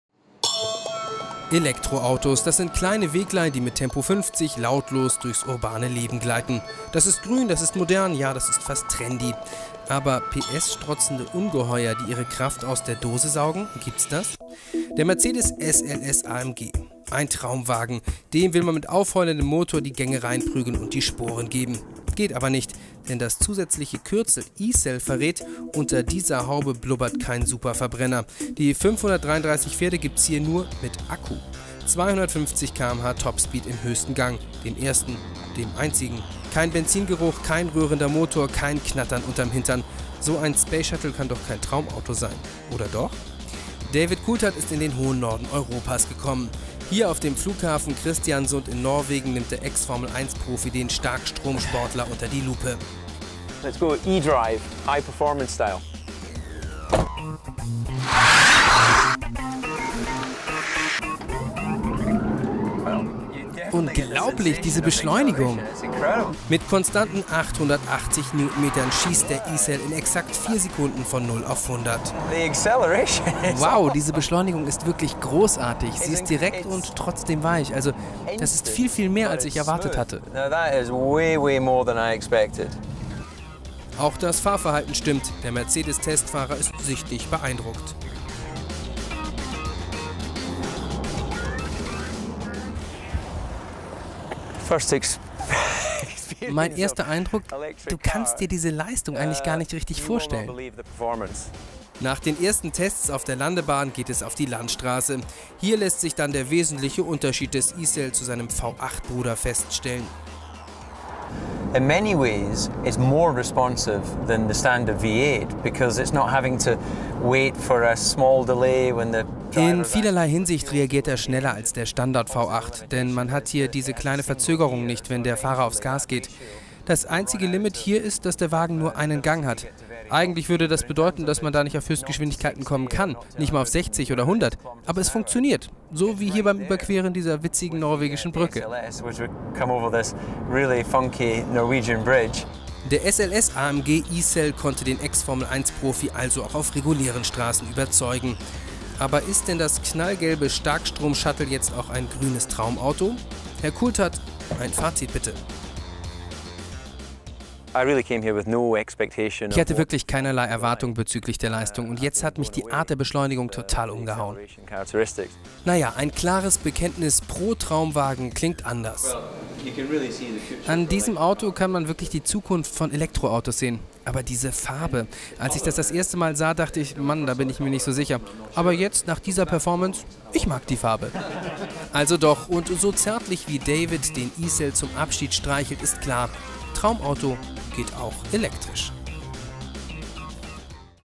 Mercedes SLS E-Cell - Coulthard im E-SLS
Mercedes setzt den SLS AMG unter Strom. Kann der E-Benz den ehemaligen Formel-1-Pilot David Coulthard überzeugen? AUTO BILD TV war bei der Testfahrt dabei.